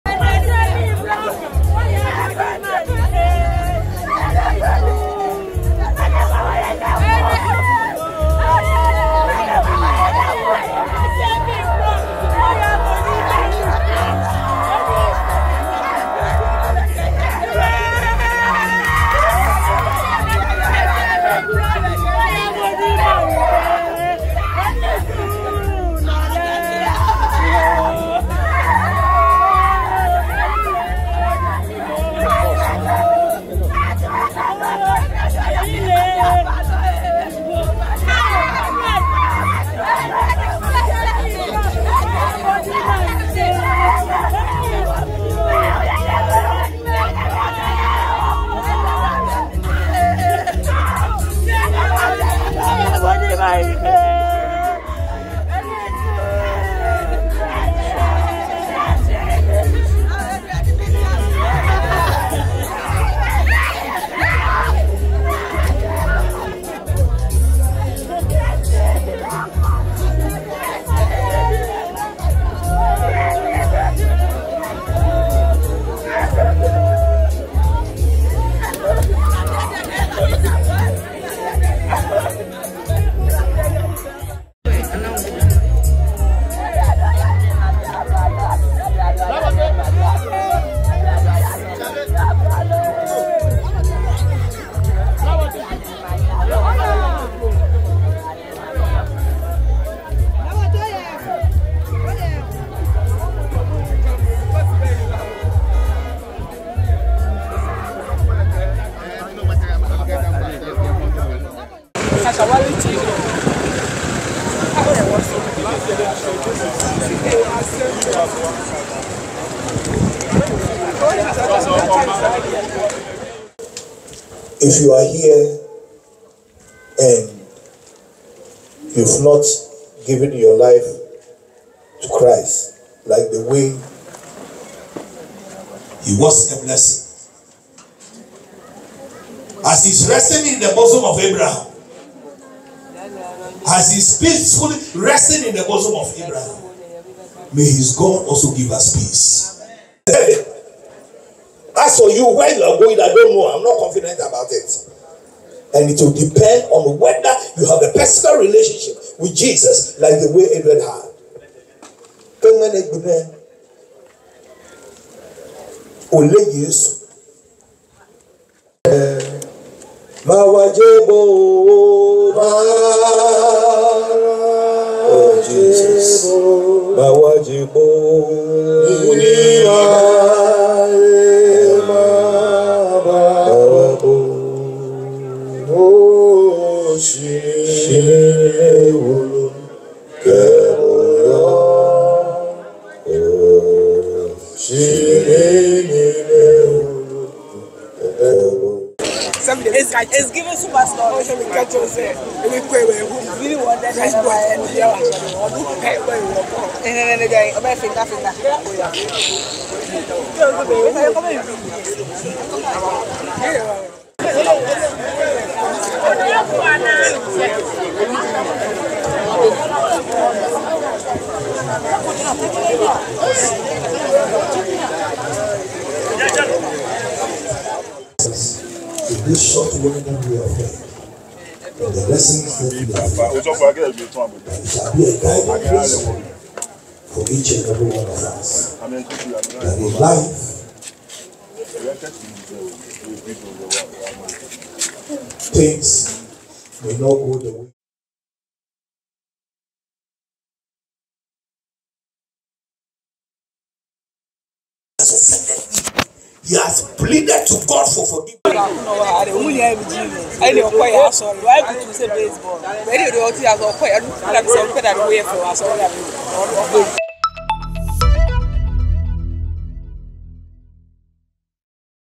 Emotional scenes at the funeral service of the deceased O’Reilly student || 3NewsGH
Emotional scenes at the funeral service of the deceased O’Reilly student